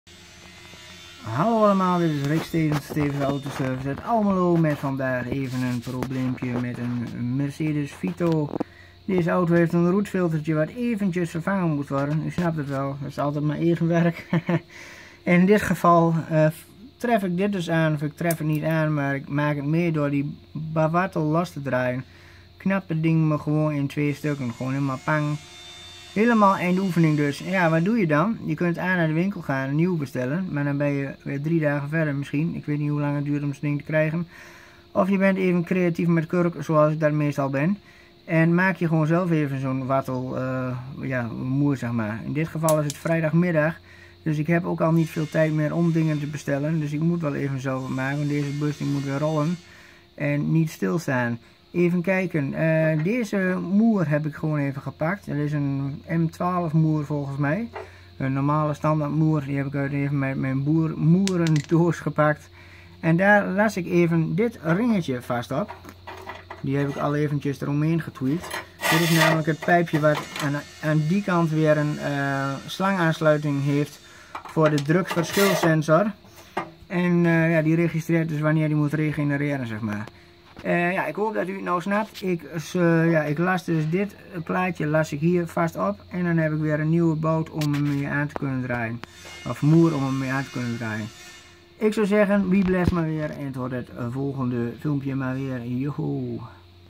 Roetfilter Wartel van druk pijpje doormidden geknapt op vrijdag middag en je klant moet rollen...
Mercedes Vito /Viano, VW, op 90% van de roetfilters zit deze constructie...
Vrijdagmiddag, net te laat om nog een nieuwe wartel/ drukverschil pijp te krijgen, dus maken we er gewoon zelf een met een M12 moer en plaatje... 😅
foutcodes: stuk of 10 roetfilter gerelateerd.
drukverschil sensor, regenereren onmogelijk enzovoort enzovoort sissend geluid onder auto...